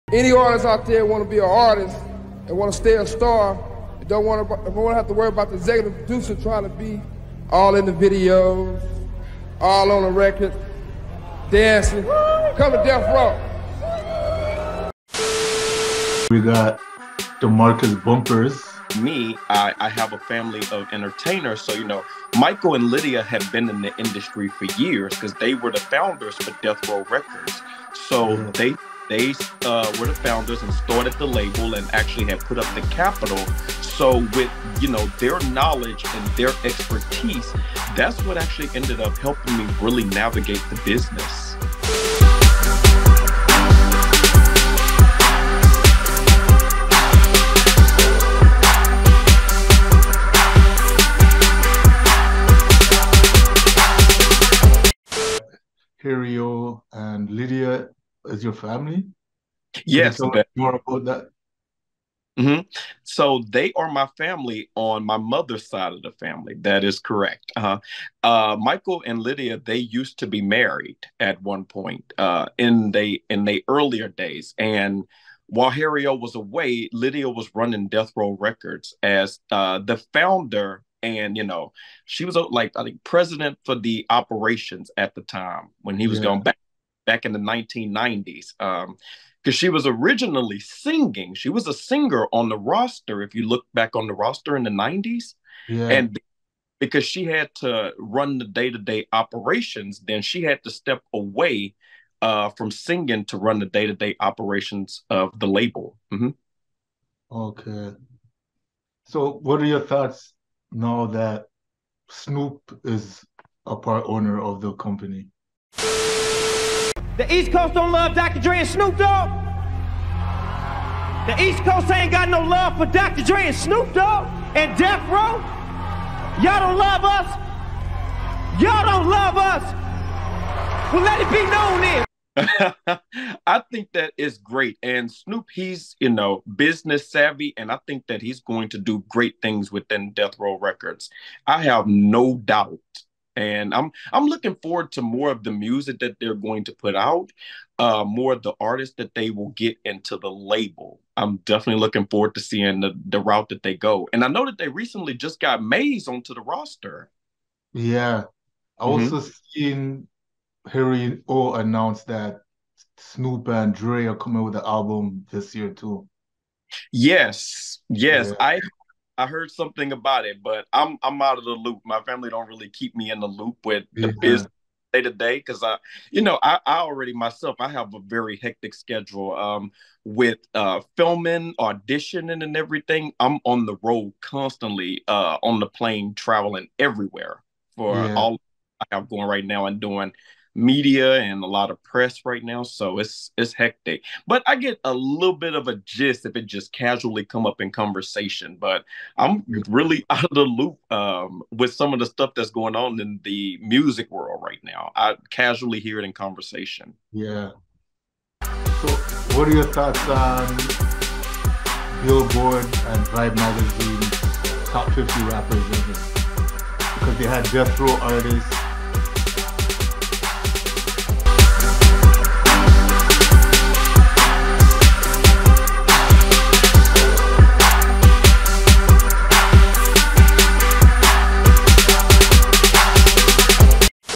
Death Row Records Family Member Demarcus Bumpers On Harry O & Snoop Dogg Ownership Exclusive
Death Row Records family member Demarcus Bumpers gives WorldWide Entertainment TV Media exclusive insight into the origins of the infamous record label that dominated 90s hip hop.

Boasting a roster that consisted of Tupac Shakur, Dr. Dre, Snoop Dogg, Nate Dogg, and Tha Dogg Pound, fans all over the world have memories of their classic hit songs.

We discover that Harry O is the true financial backer of the label and the plans for the 2023 new album from Snoop and Dr. Dre. Check out the first installment of this interview in the video above.